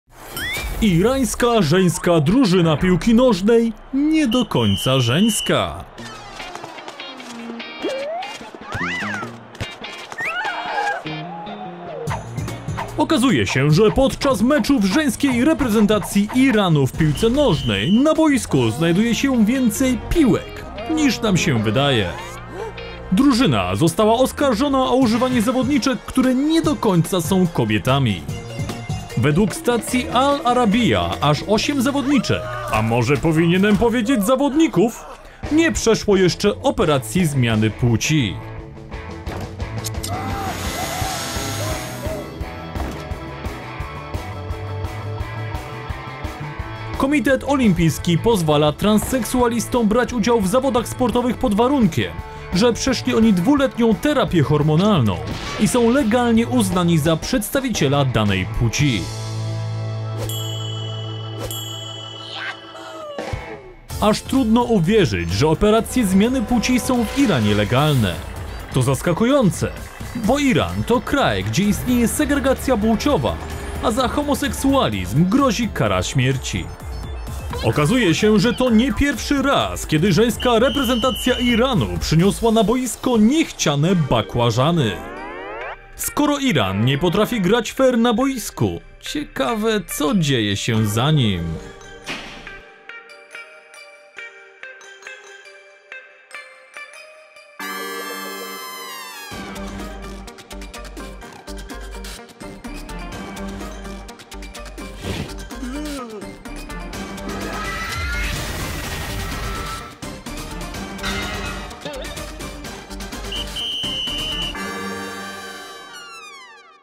Irańska żeńska drużyna piłki nożnej nie do końca żeńska...
Okazuje się, że podczas meczów żeńskiej reprezentacji Iranu w piłce nożnej na boisku znajduje się więcej piłek niż nam sie wydaje...
Drużyna została oskarżona o używanie zawodniczek, które nie do końca są kobietami.
Według stacji Al Arabiya aż 8 zawodniczek...a może powinienem powiedizec zawodników,  nie przeszło jeszcze operacji zmiany płci.
Komitet Olimpijski pozwala transseksualistom brać udział w zawodach sportowych pod warunkiem, że przeszli oni 2-letnią terapię hormonalną i są legalnie uznani za przedstawiciela danej płci.
Aż trudno uwierzyć, że operacje zmiany płci są w Iranie legalne. To zaskakujące, bo Iran to kraj gdzie istnieje segregacja płciowa, a za homoseksualizm grozi kara śmierci.
Okazuje się, że to nie pierwszy raz kiedy żeńska reprezentacja Iranu przyniosła na boisko niechciane bakłażany...
Skoto Iran nie potrafi grać fair na boisku, ciekawe co dzieje się poza nim...?